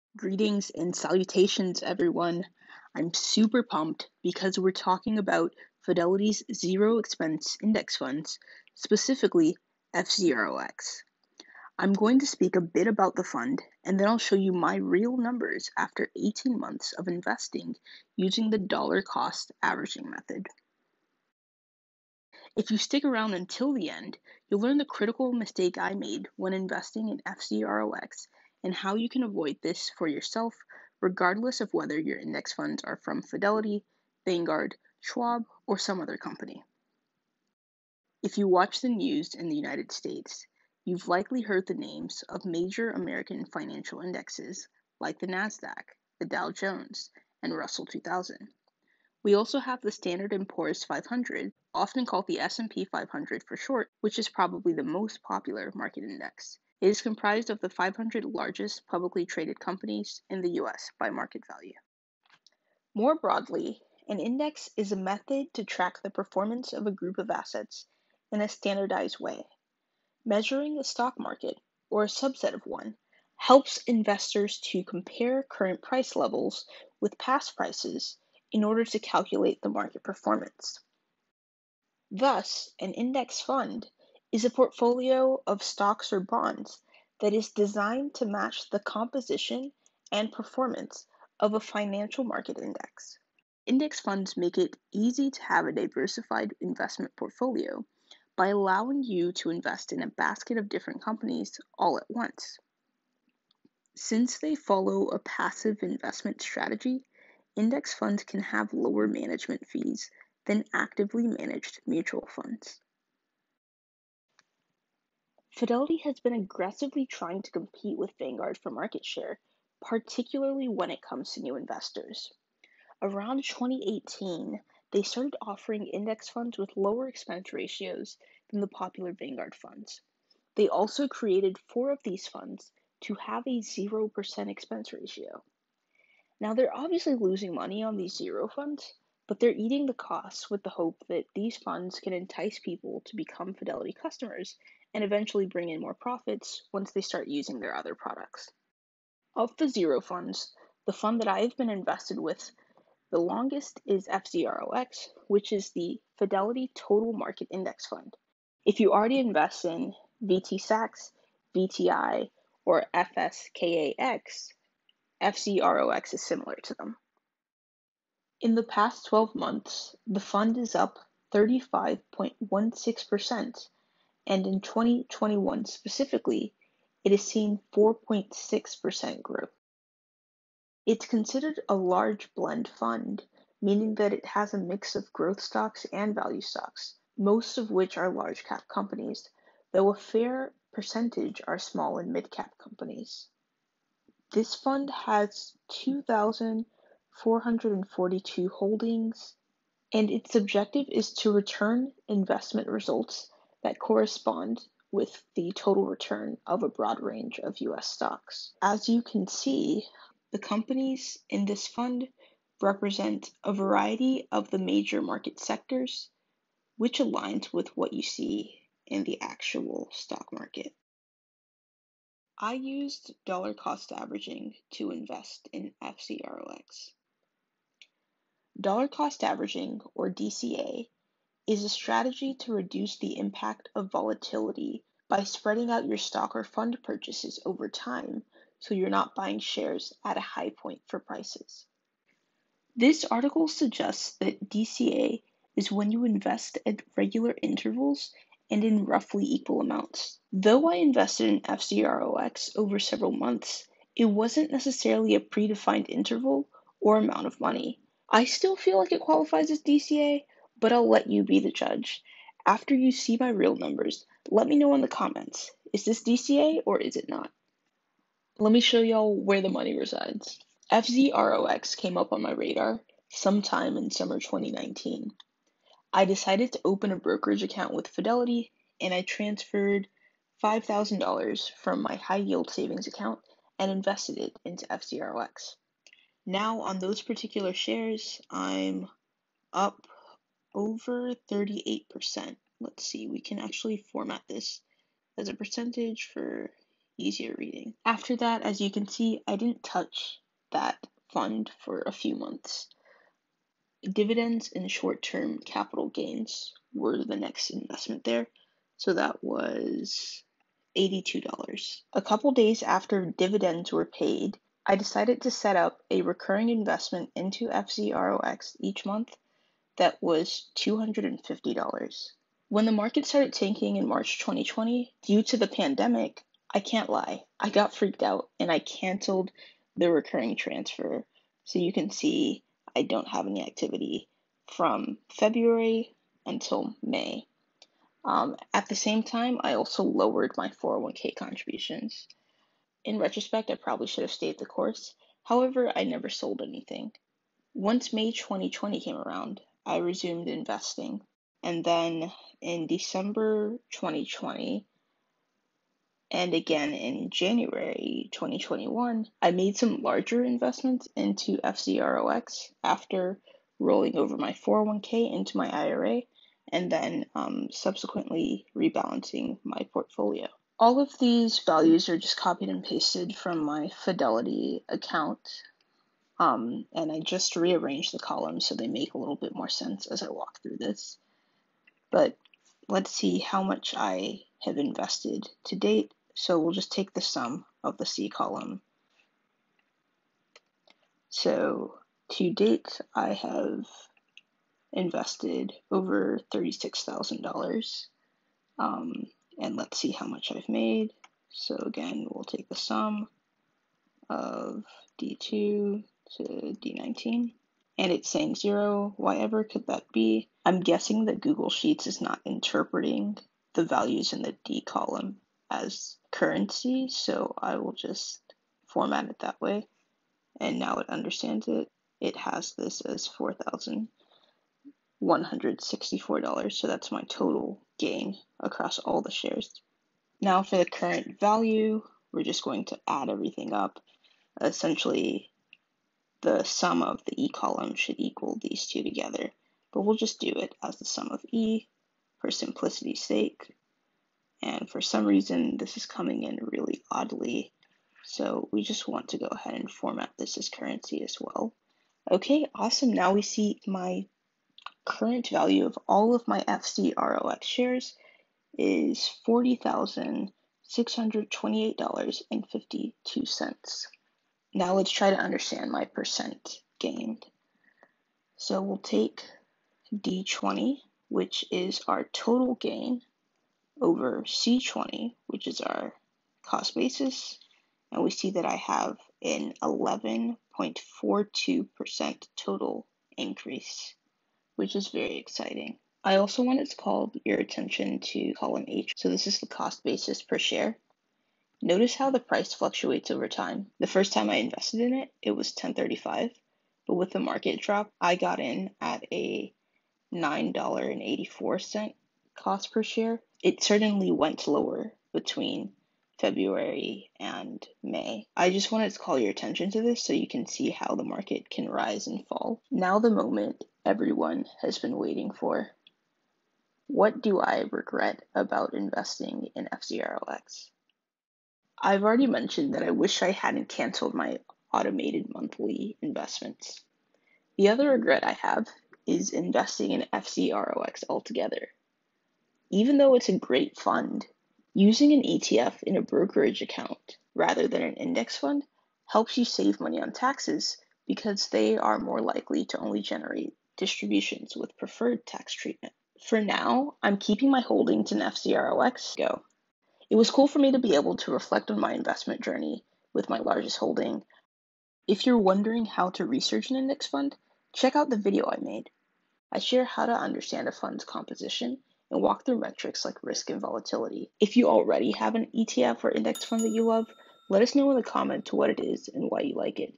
FZROX Investing | The Good, Bad, and Ugly | Financial Independence
FZROX Investing | The Good, Bad, and Ugly |  My Journey to Financial Independence

Today we’re talking about the Fidelity® ZERO Total Market Index Fund (FZROX) and my experiences investing over the past 18 months with my real numbers. 🤑 I weigh in on some dollar cost averaging controversy and share some of my investing regrets. FZROX is one of the main index funds I have invested in to help me reach financial independence and retire early.

0:00 Intro
0:38 Index Funds Defined
1:54 FZROX
3:47 Dollar Cost Averaging
4:27 My Investing Numbers
8:41 To Regret is Divine
9:06 Researching Index Funds

There are a number of  Index Funds & ETFs that are similar to the Fidelity® ZERO Total Market Index Fund. These include Vanguard's VTSAX and VTI funds, Fidelity's FSKAX index fund and Schwab's SWTSX index fund.

Image Attributions: Pexels, Pixabay, Freepik